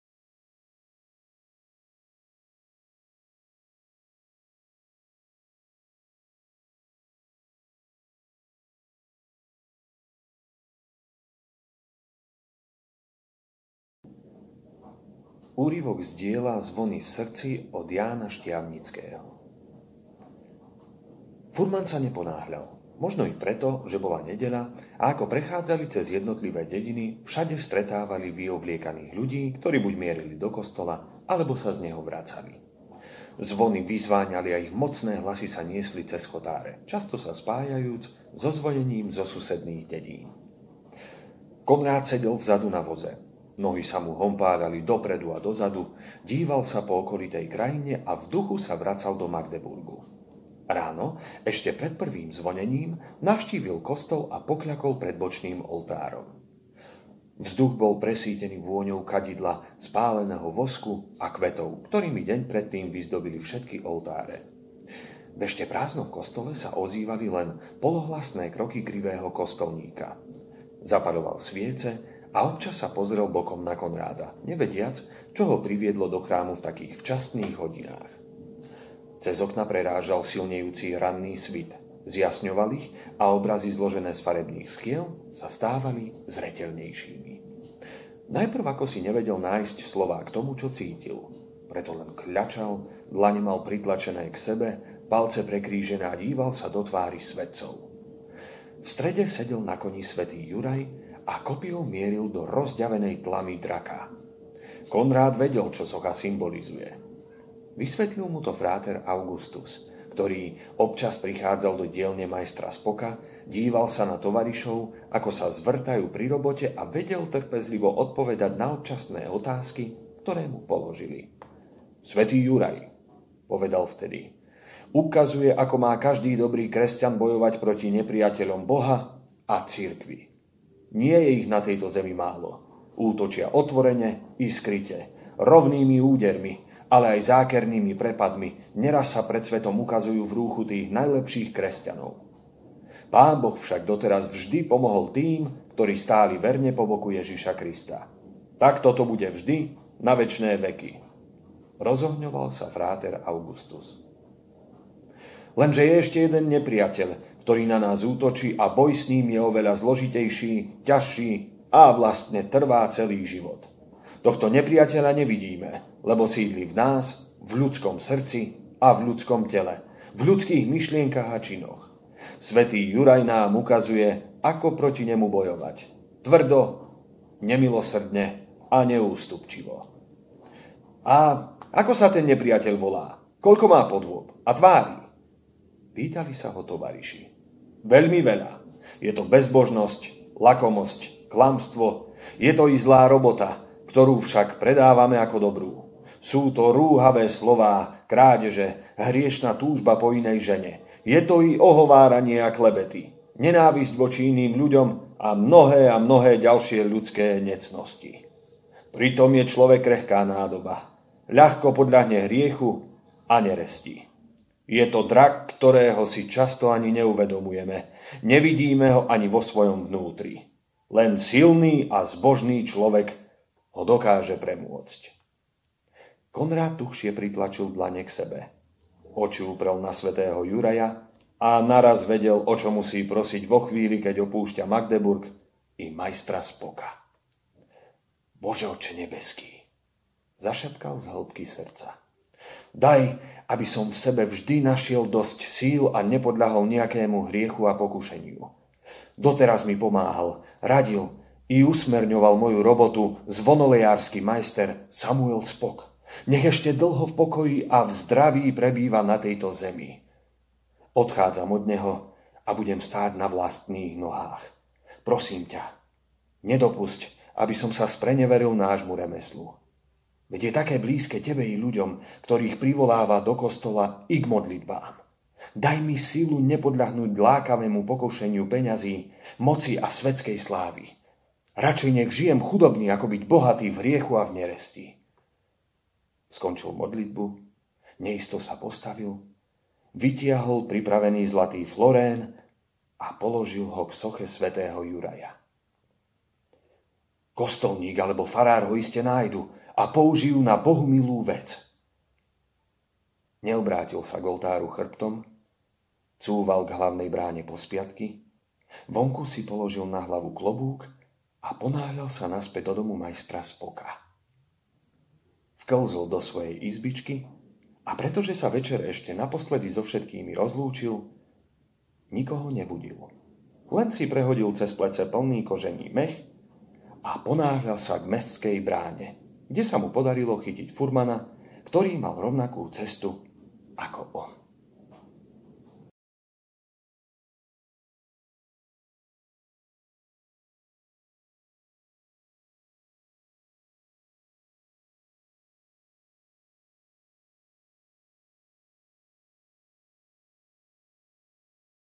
Čítanie s hercami SD: Ján Štiavnický - Zvony v srdci
Mikuláš Dzurík a Ján Štiavnický. Dnes prinášame úryvok z nádherného diela Zvony v srdci od Jána Štiavnického.

Tieto texty vyšli v domácom vydavateľstve FAMAart v Spišskej Novej Vsi, ktoré nám ich aj poskytlo – bez nároku na honorár.